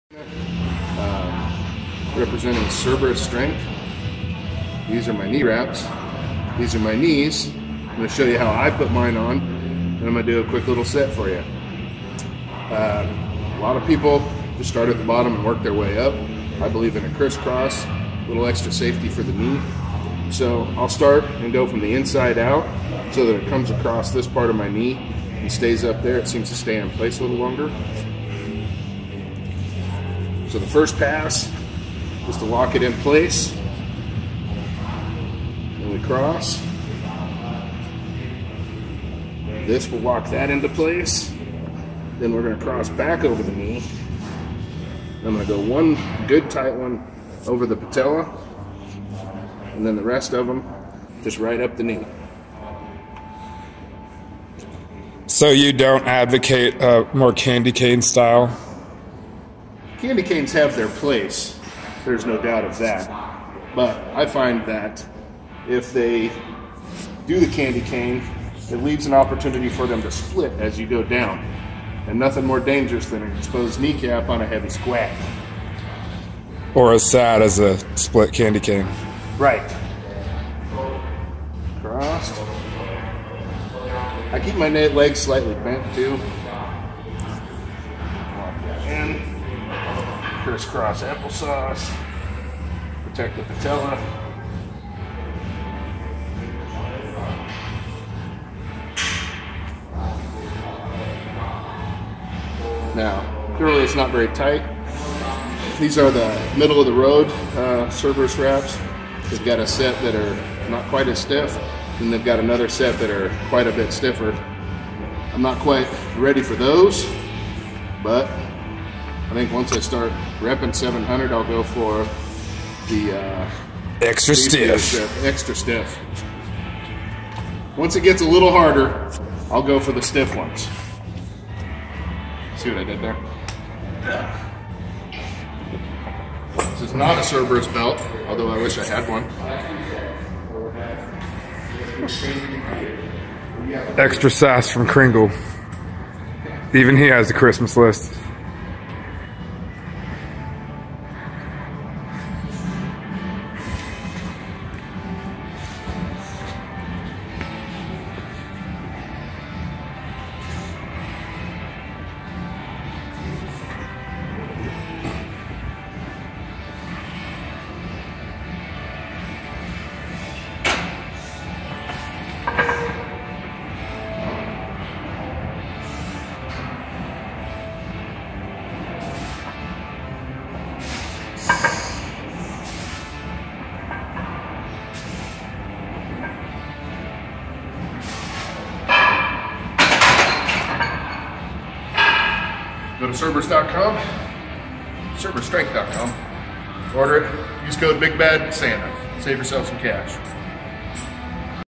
Big Bad Santa knee wrapping with Cerberus
Today I'm showing how I wrap my knees with Cerberus Strength USA knee wraps.